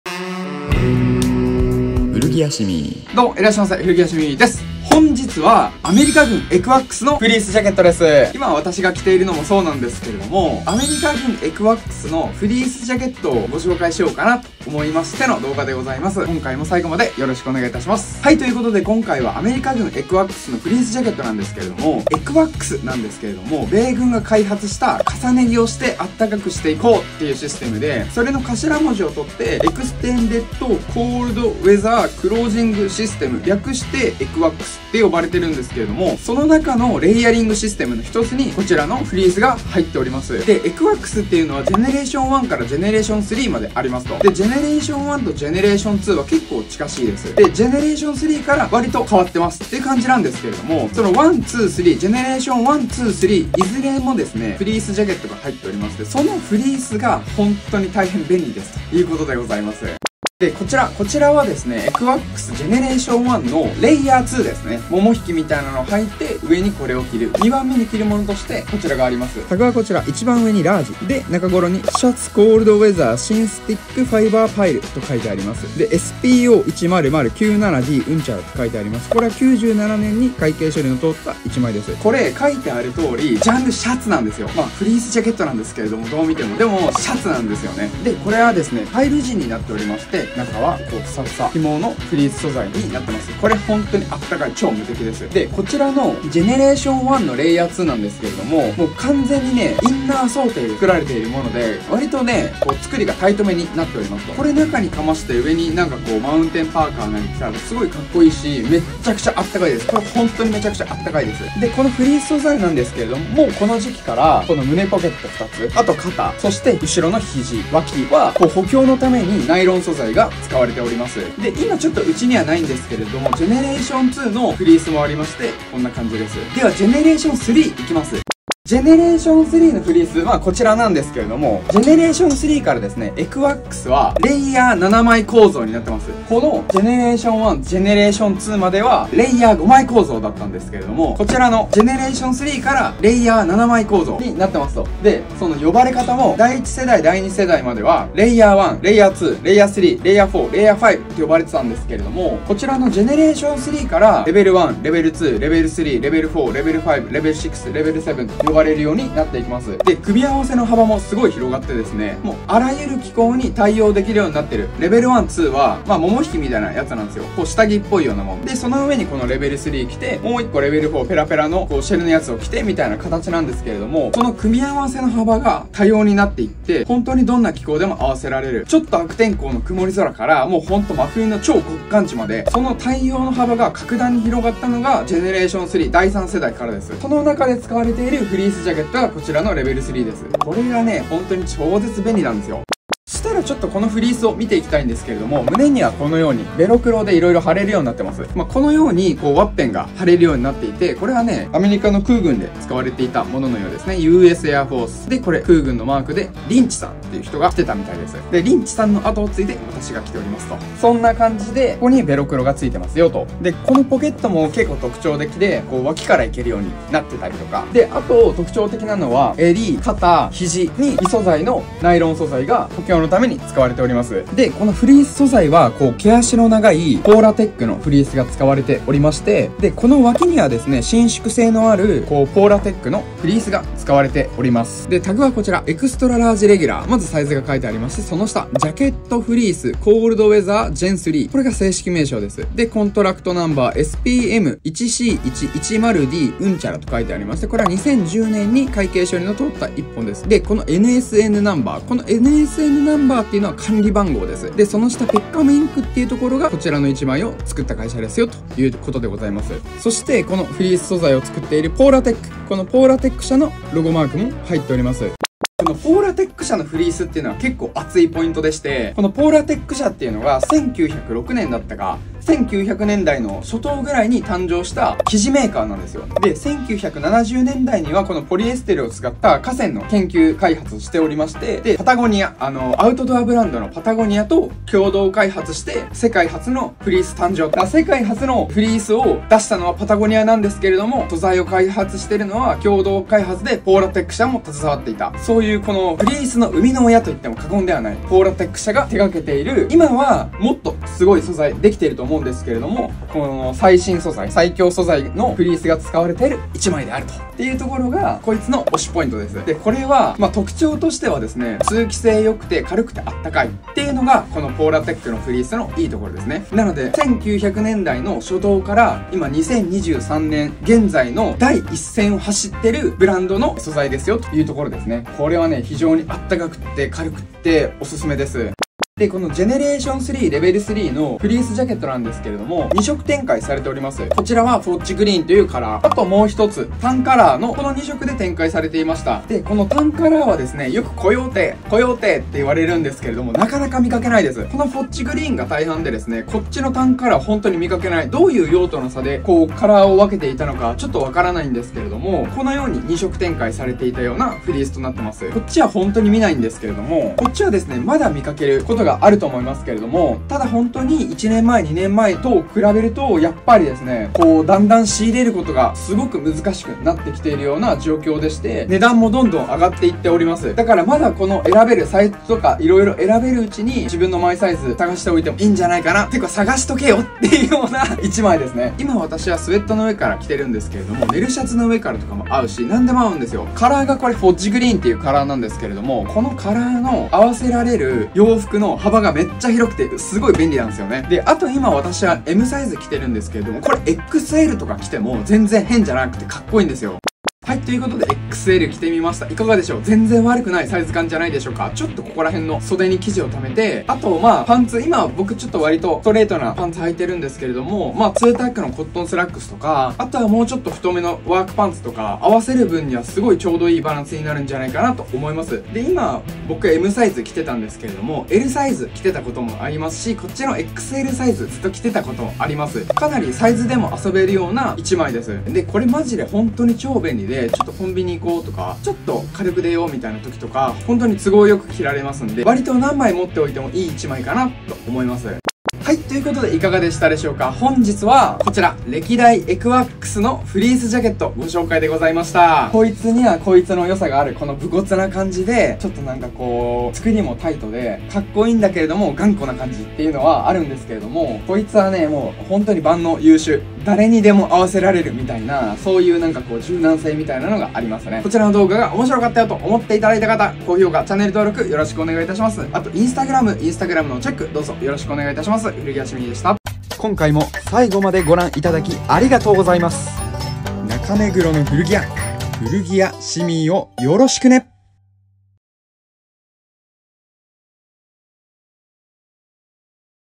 古着でフリース探すならコレっしょ！
アメリカ軍ecwcsフリースジャケット！
古着のフリースの中でも大好きなフリースです！

お店はこちら！
お気軽に遊びに来てください〜！
【場所】目黒区上目黒2-14-2
【時間】月-金14-21 土12-21 日12-20

中目黒でお待ちしております！